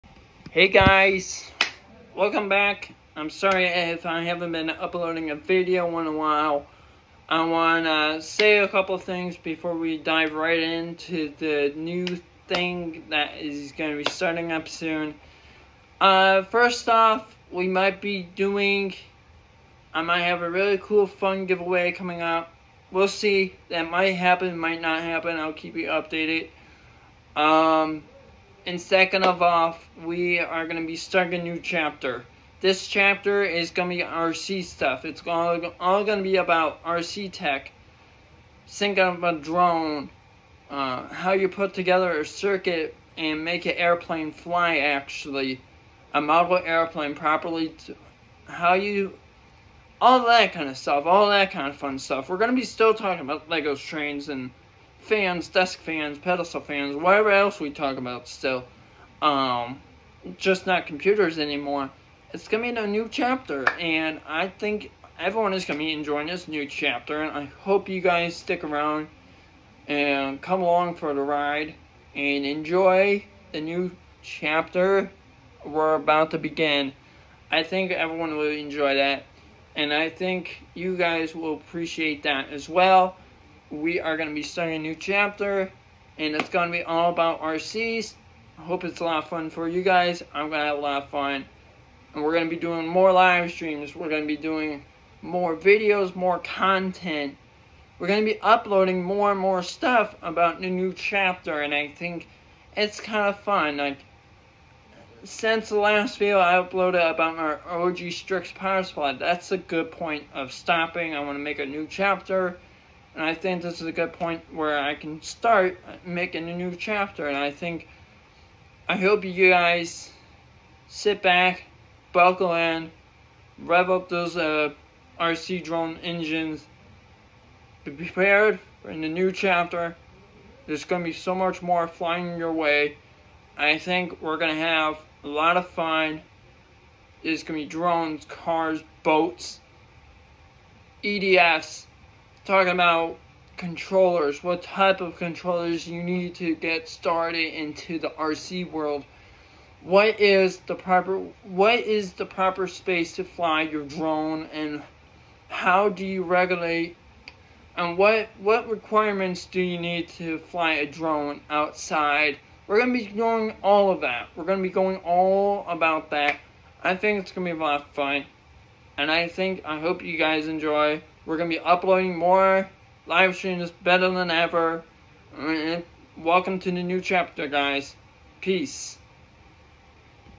NEW CHARACTER
Hey guys, we’re gonna be starting a new chapter start up those RC engines just gonna be a lot more videos flying your way, and I hope you enjoy this new world that we’re gonna get into